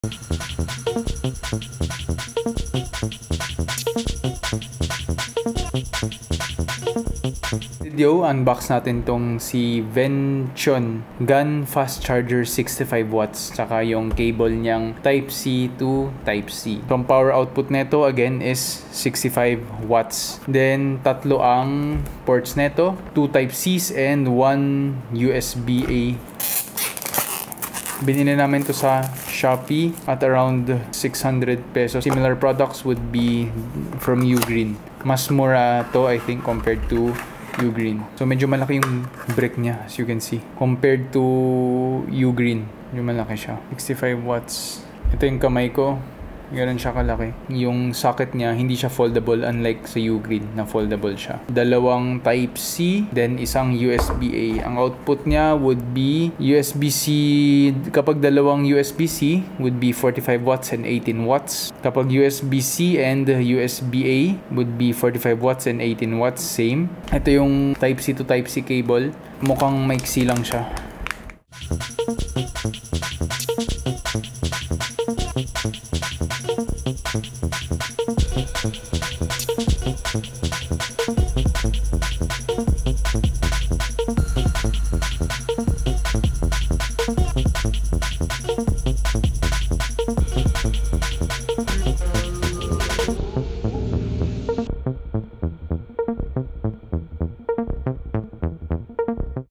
VENTION GAN 65W FAST CHARGER | IPHONE SAMSUNG TABLET MACBOOK | TAGALONG UNBOXING TEST
PRODUCT SPECIFICATIONS:
Quick Charge 4.0 + 2 Type C 1 USB C PD Ports
Input: 100-240V～50/60Hz 1500mA
Output（USB-C1）: 5V/3A,9V/3A,12V/3A,15V/3A,20V/3.25A
Output（USB-C2）: 5V/3A,9V/3A,12V/2.5A,15V/2A,20V/1.5A
Output（USB）:4.5V/5A,5V/4.5A,9V/3A,12V/2.5A,20V/1.5A

Multi-port Output:
USB-C1+USB-C2:45W+18W
USB-C1+USB:45W+18W
USB-C2+USB:15W(5V/3A)

Support protocol:
USB-C1：PPS/PD 3.0/QC 4+/SCP/FCP/AFC/MTK PE/BC 1.2/Apple 2.4A
USB-C2：PPS/PD 3.0/QC 4+/SCP/FCP/AFC/MTK PE/BC 1.2/Apple 2.4A
USB-A：QC 3.0/SCP/FCP/AFC/BC 1.2/Apple 2.4A